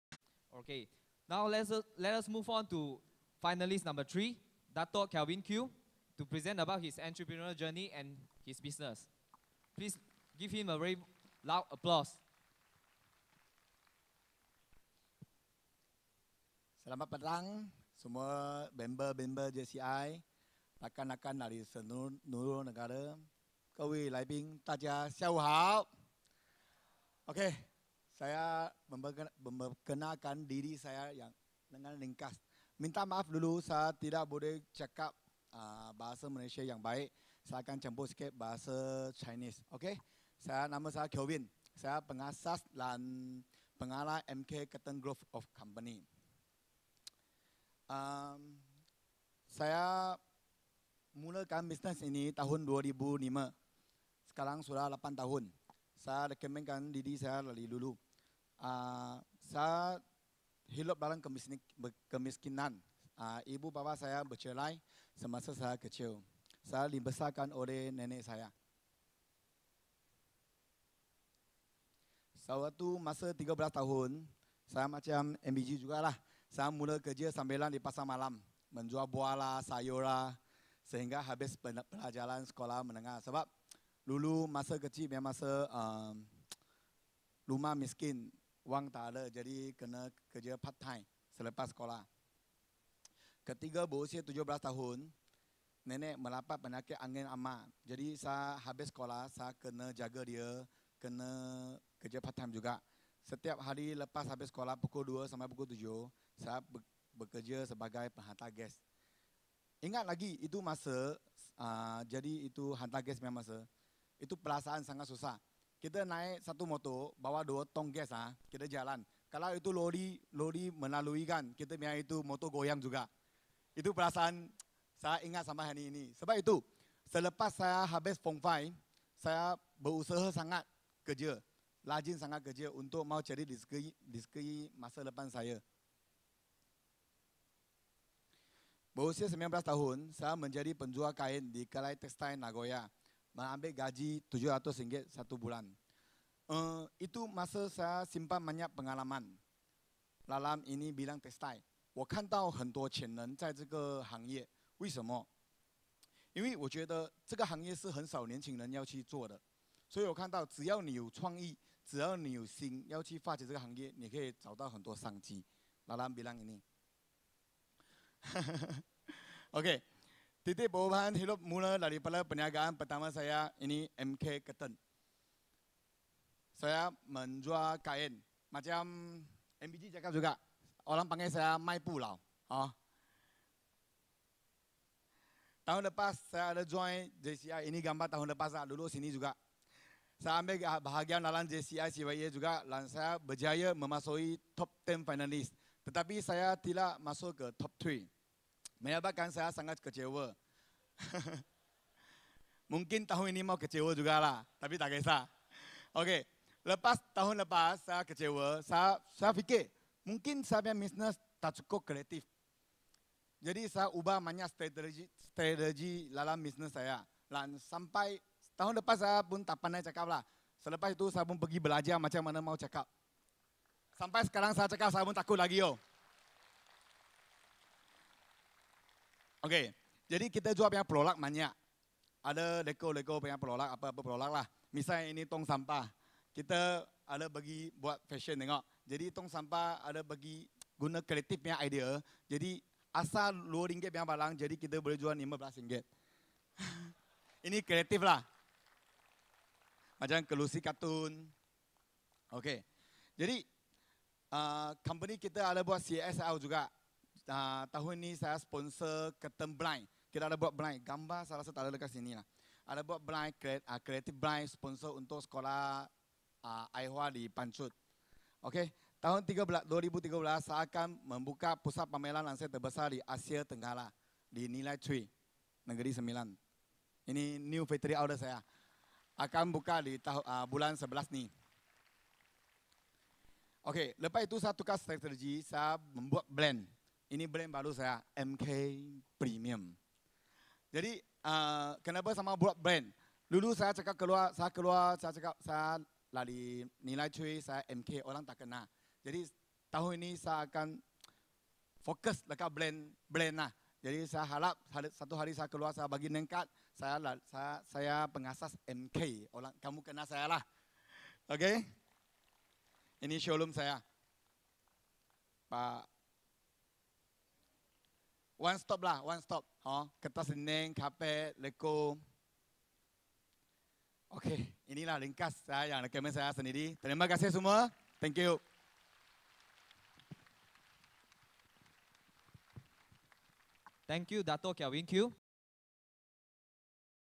8 of 24: Dato' Calvin Khiu @ MK Curtain Group sharing @ Plenary Hall KLCC 2pm 07Sept 2013 JCI CYEA
8 of 24: Dato' Calvin Khiu @ MK Curtain Group Entrepreneur sharing @ Plenary Hall, Kuala Lumpur Convention Centre, KLCC @ 2pm- 07September 2013 ~ 2013 JCI CYEA TOP 10 Entrepreneur Sharing Session